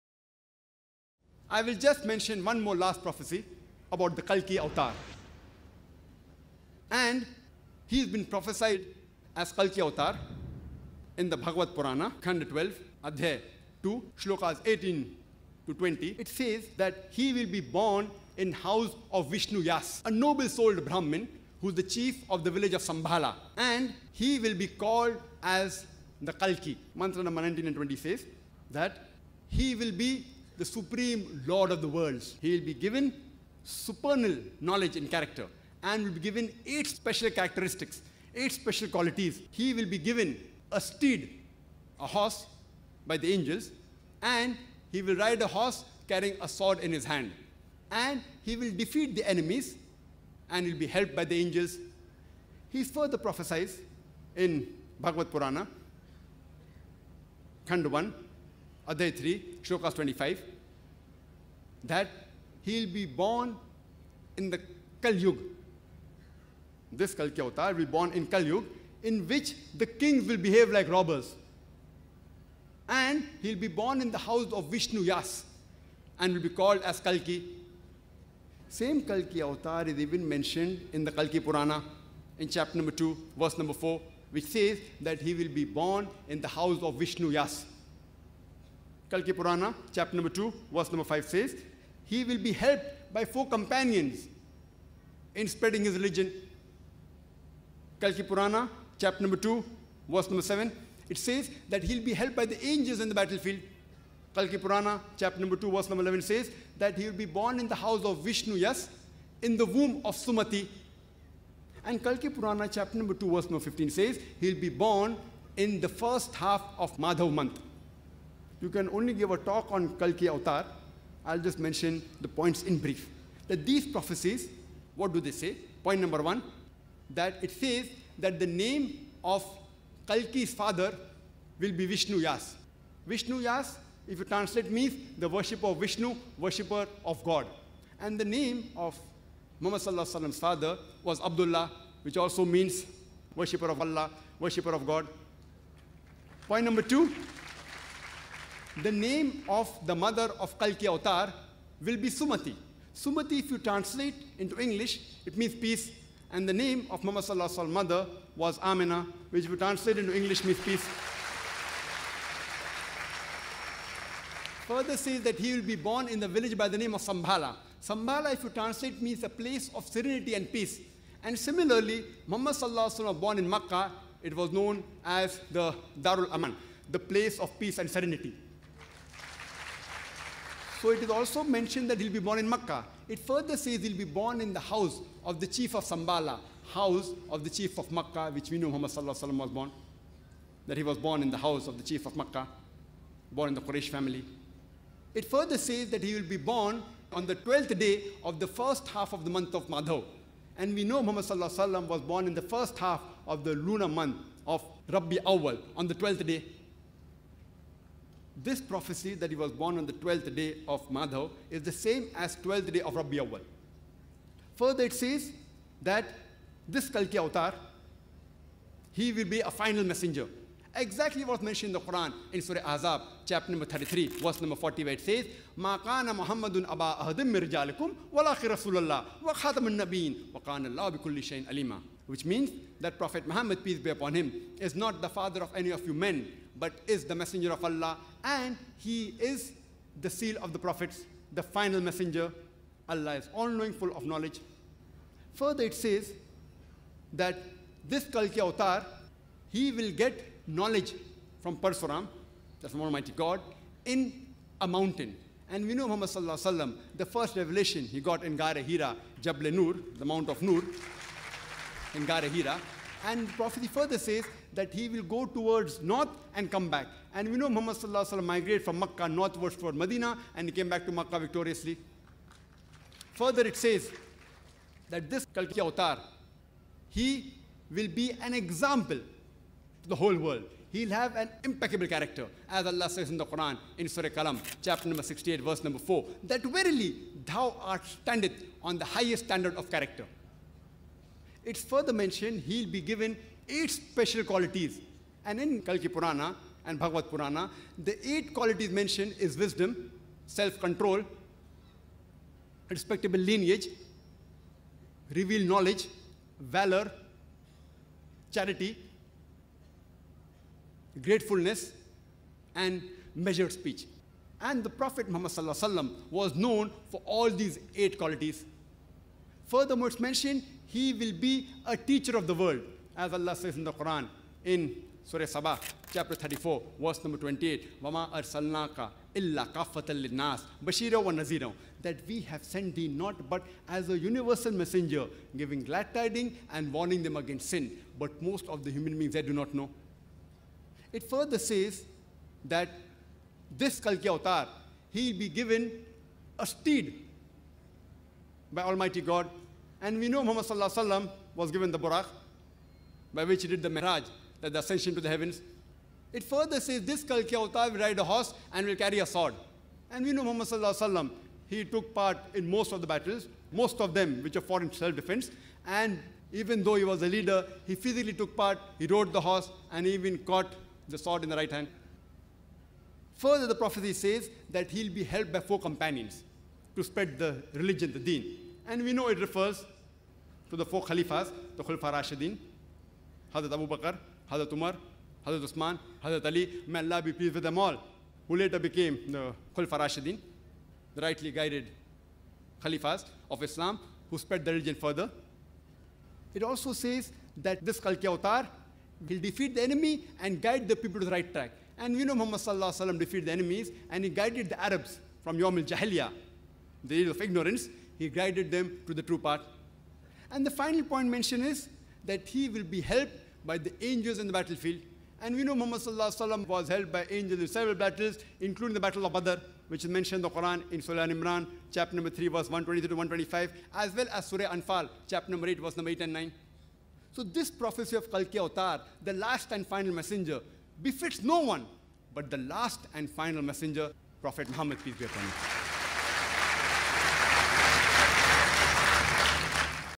The Final Avatar of Hindus | Kalki Avatar - Could it be the Prophet of the Muslims 👆
Join renowned scholar Dr. Zakir Naik as he delves into the fascinating topic of Prophet Muhammad in Hindu scriptures, specifically the concept of Kalki Avatar. In this thought-provoking video, explore the intriguing possibility of Prophet Muhammad being the Kalki Avatar as discussed by Dr. Zakir Naik. Discover the connections and interpretations between Hinduism and Islam, as we unravel the prophetic traditions and eschatological beliefs. Engage in a Hindu-Muslim dialogue that fosters understanding and unity, shedding light on the shared values and teachings. Don't miss this insightful exploration of the Kalki Avatar and its potential connection to Prophet Muhammad, as we seek to bridge the gap and promote interfaith harmony.